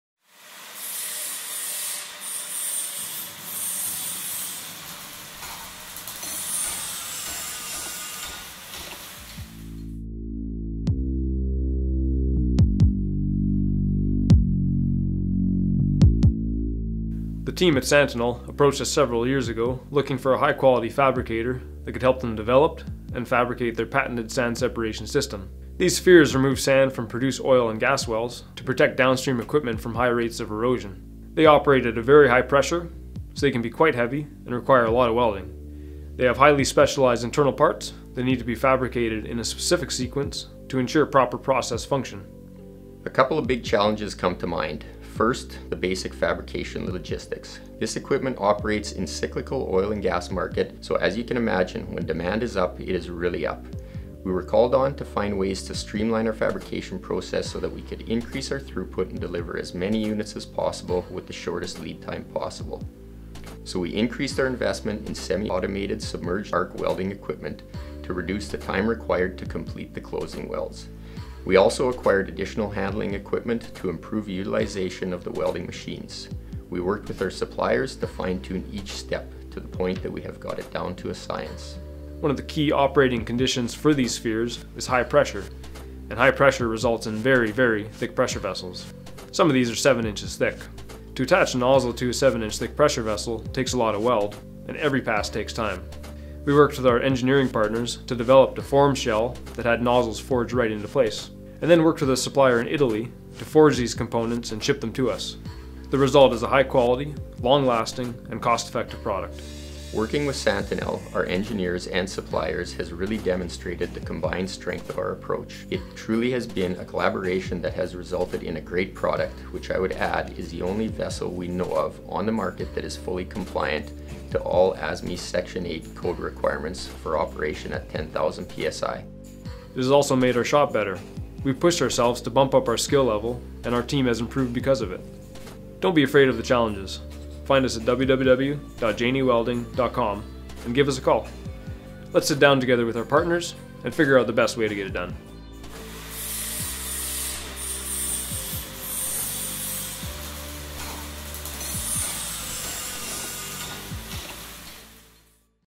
Reliable High Capacity Pressure Vessel Fabrication
See how JNE Welding delivered large numbers of high pressure separator vessels to our customer for oil & gas producers across North America from our shop in Saskatoon, Saskatchewan, Canada.